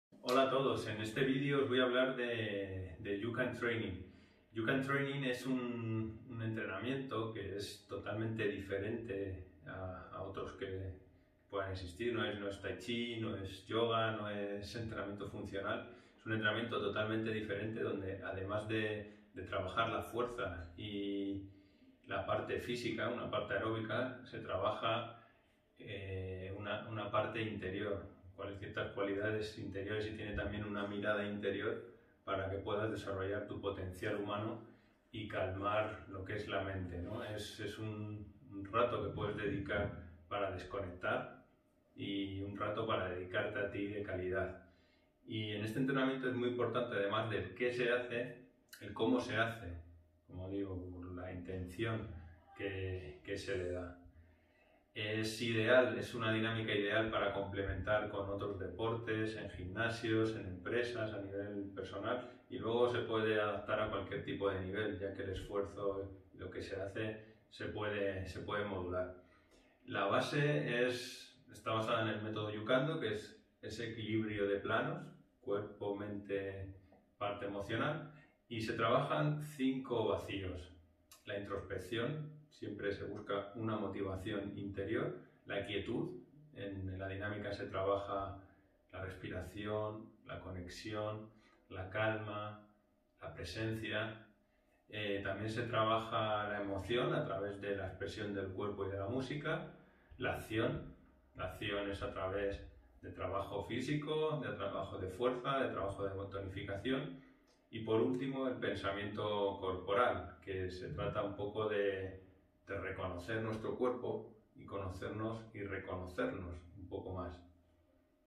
YUCANTRAINING Entrenamiento consciente
En este vídeo explicamos en que consiste el entrenamiento consciente YUCANTRAINING